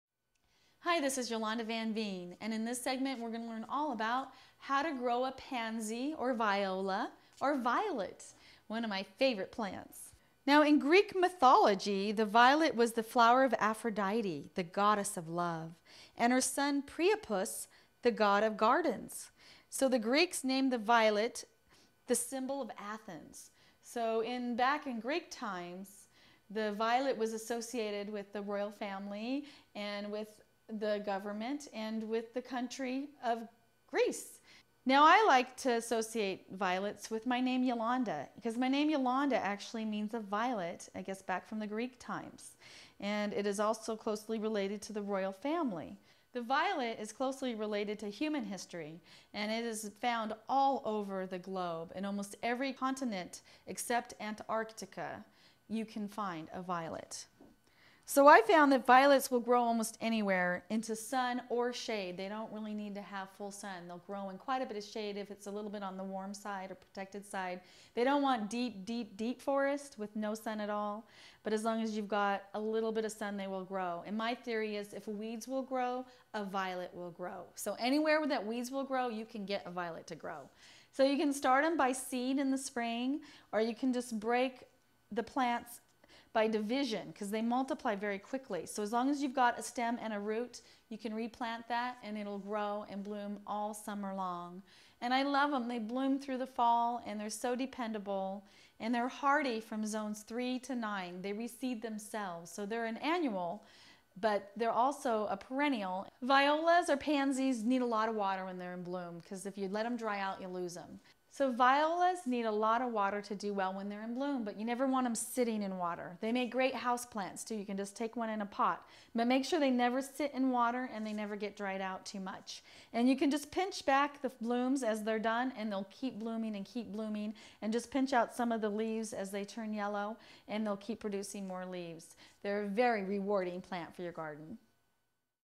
Flower Gardening Tips : How to Grow Pansy/Viola (Viola)
The pansy, viola or violet is a plant that is found in almost every continent, except for Antarctica. Grow violets in the sun or shade with instructions from a sustainable gardener in this free video on gardening and plant care.